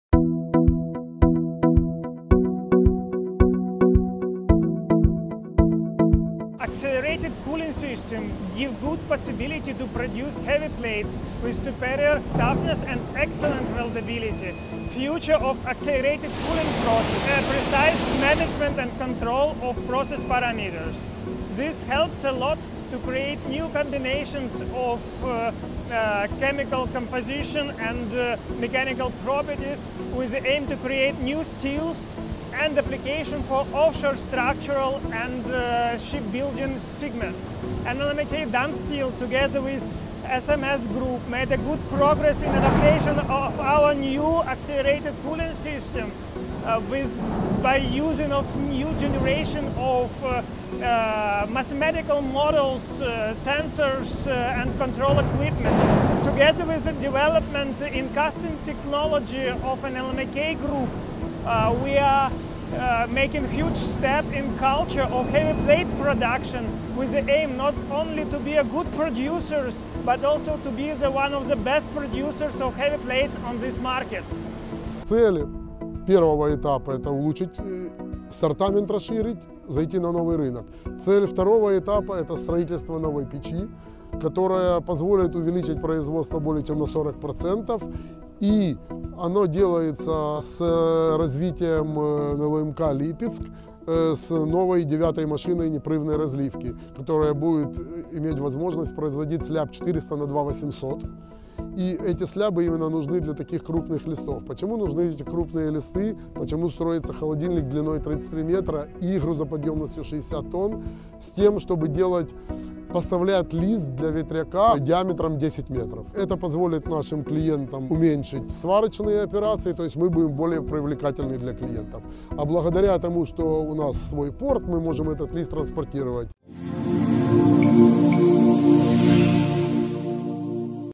NLMK DanSteel расширяет сортамент нишевого толстого листа
Группа НЛМК начала горячие испытания системы ускоренного охлаждения металла на толстолистовом стане горячей прокатки датского предприятия NLMK DanSteel. Новое оборудование позволит качественно изменить сортамент и увеличить долю производства нишевого толстого листа, в том числе для офшорной ветроэнергетики и производства сосудов под давлением.